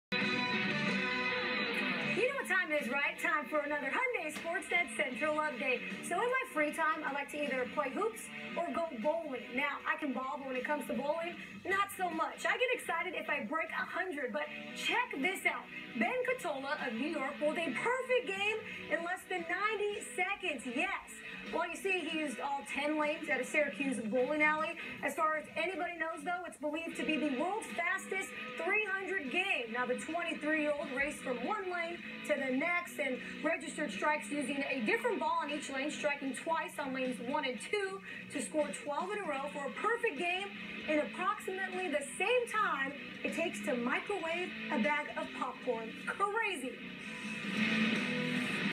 Man bowls a 300 game in 90 seconds
A man bowls a perfect  game in just 90 seconds, as he quickly goes down the alley from lane to lane heaving a perfect strike 12 consecutive times!
Incredible Bowling